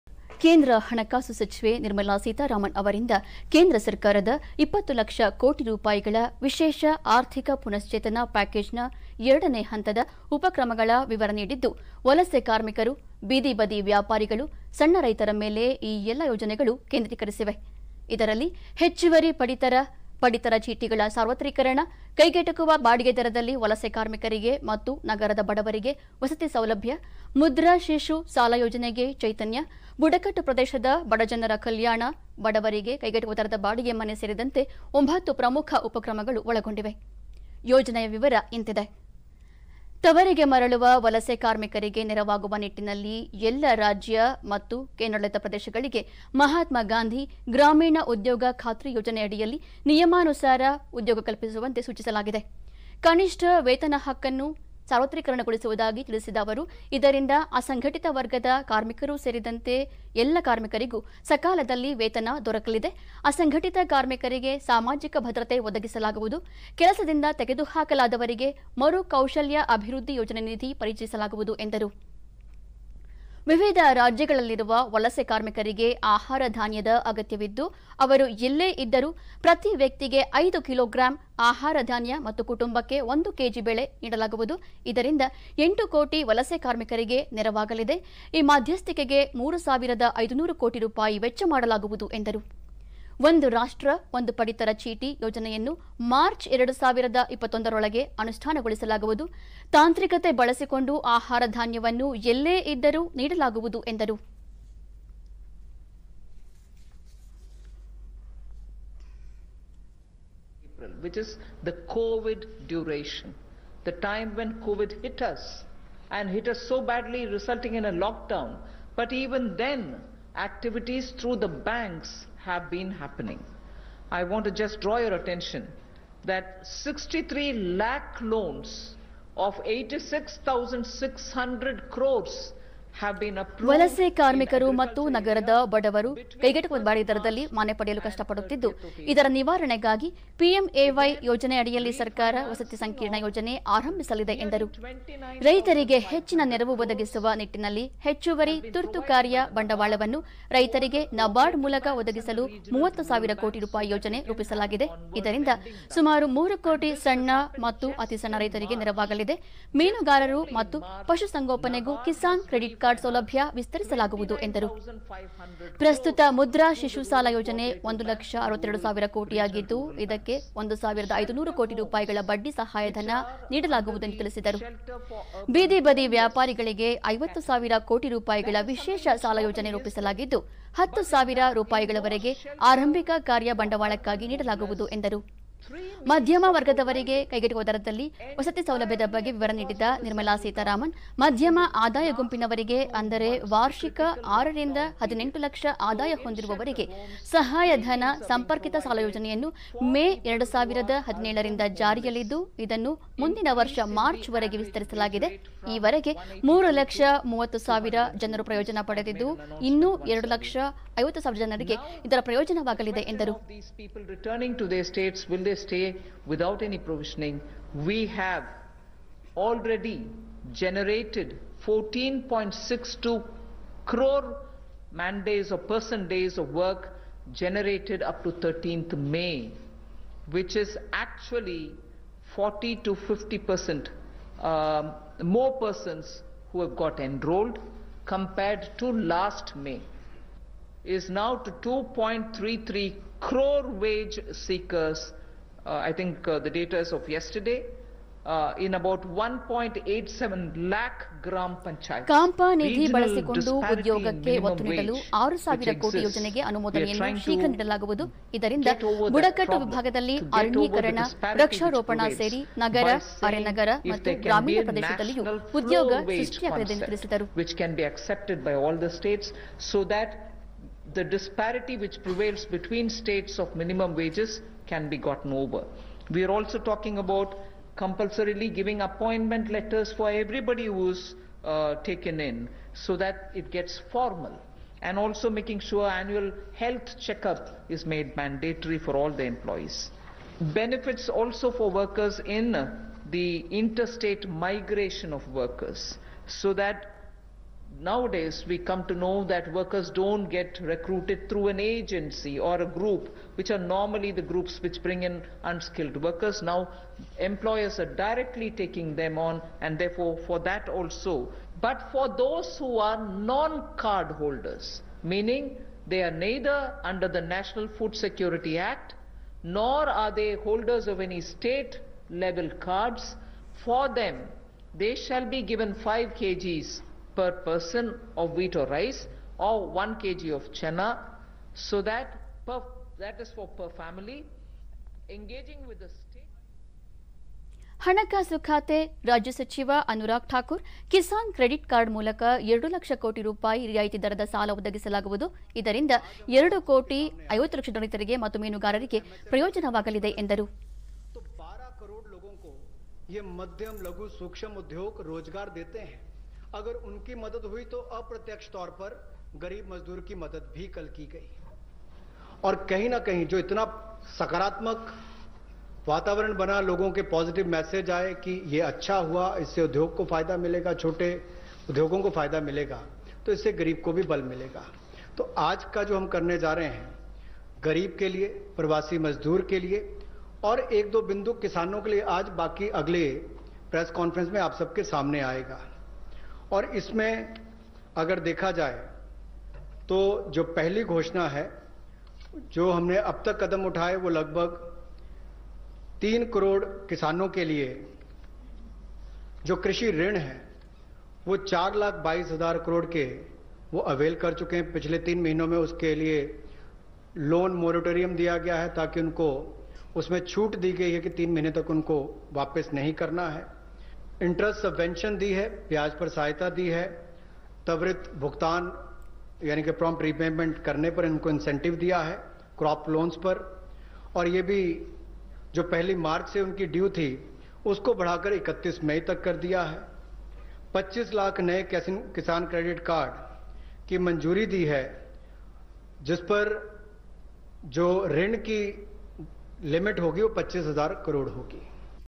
20 ಲಕ್ಷ ಕೋಟಿ ಪ್ಯಾಕೇಜ್ ಬಗ್ಗೆ ಮತ್ತಷ್ಟು ವಿವರ ನೀಡಿದ ಸಚಿವೆ ನಿರ್ಮಲಾ ಸೀತಾರಾಮನ್
ಕೇಂದ್ರ ಹಣಕಾಸು ಸಚಿವೆ ನಿರ್ಮಲಾ ಸೀತಾರಾಮನ್ ಅವರಿಂದ ಕೇಂದ್ರ ಸರ್ಕಾರದ 20 ಲಕ್ಷ ಕೋಟಿ ರೂಪಾಯಿಗಳ ವಿಶೇಷ ಆರ್ಥಿಕ ಪುನಶ್ಚೇತನ ಪ್ಯಾಕೇಜ್ ನ ಎರಡನೇ ಹಂತದ ಉಪಕ್ರಮಗಳ ವಿವರ ನೀಡಿದ್ದು, ವಲಸೆ ಕಾರ್ಮಿಕರು, ಬೀದಿ ಬದಿ ವ್ಯಾಪಾರಿಗಳು, ಸಣ್ಣ ರೈತರ ಮೇಲೆ ಈ ಎಲ್ಲ ಯೋಜನೆಗಳೂ ಕೇಂದ್ರೀಕರಿಸಿವೆ.